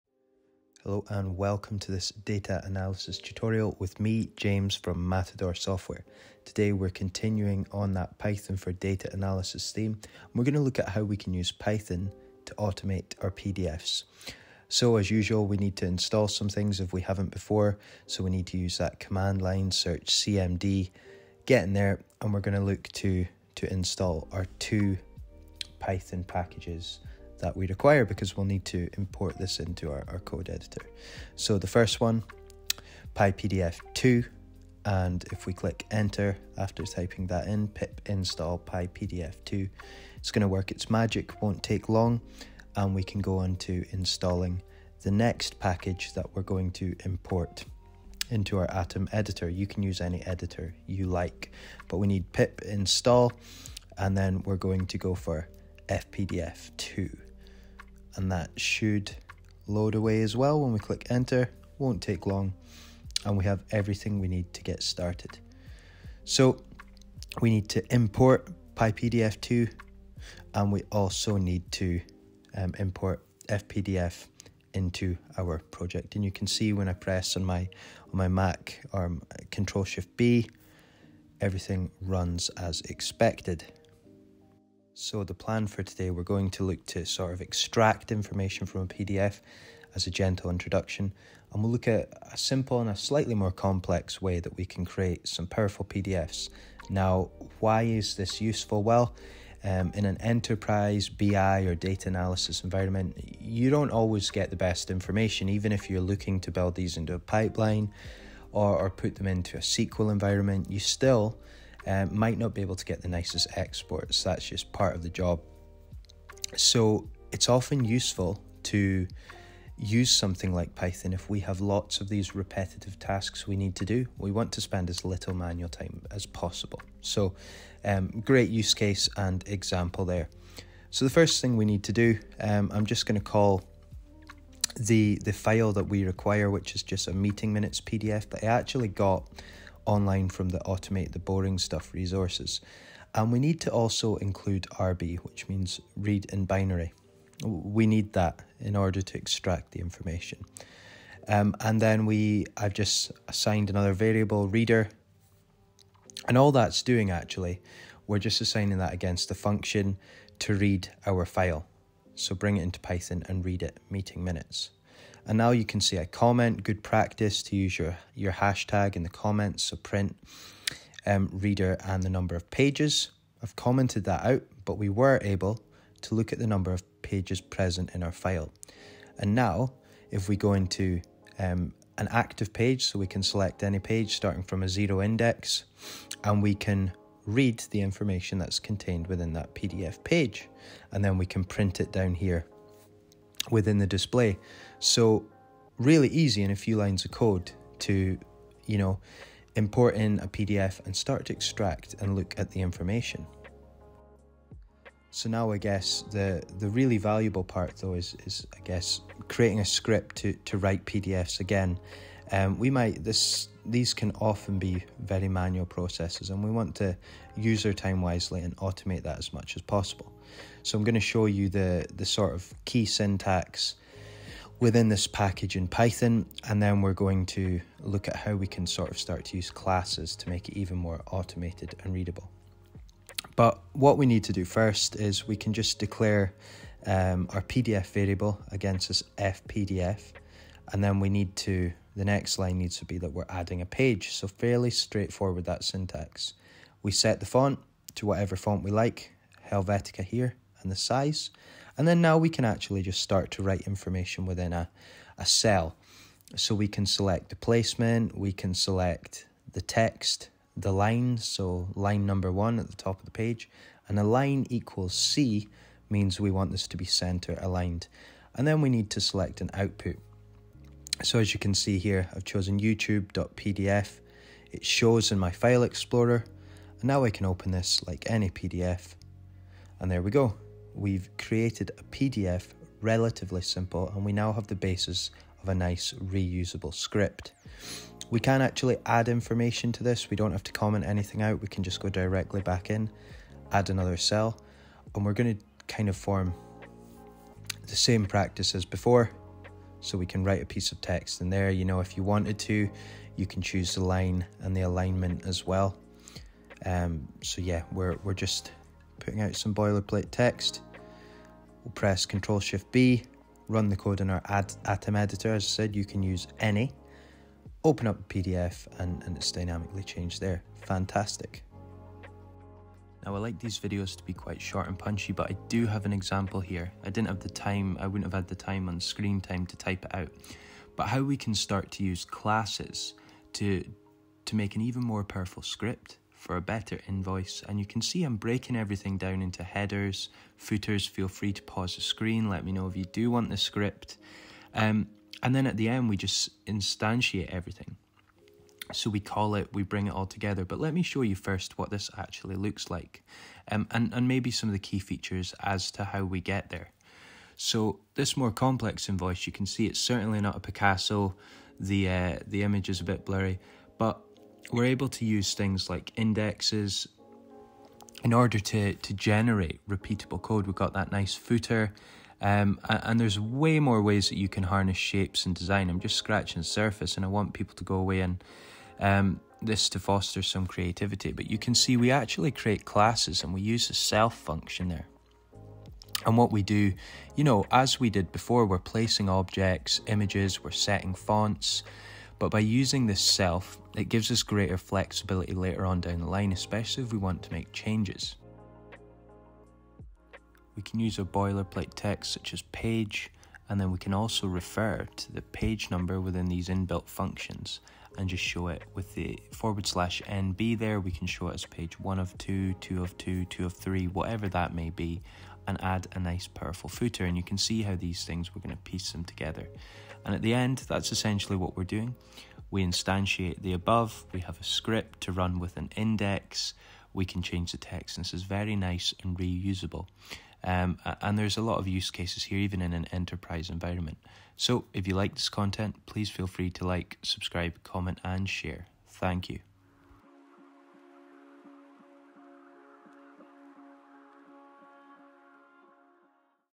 Automating PDF Files with Python | Python for Data Analysis
In this quick video, we'll be looking at how we can use Python to Automate PDF Files. We can replace repetitive and time consuming data extraction & PDF creation with convenient and flexible Python Scripts.

I'll demo how we can count sheets, extract text from selected PDF pages, create a simple PDF & how we can use Python Classes to create more powerful and flexible PDF Invoices, for example!

This is all under 10 minutes and perfect for Data Analysts, Power BI Developers or anyone looking to improve their efficiency!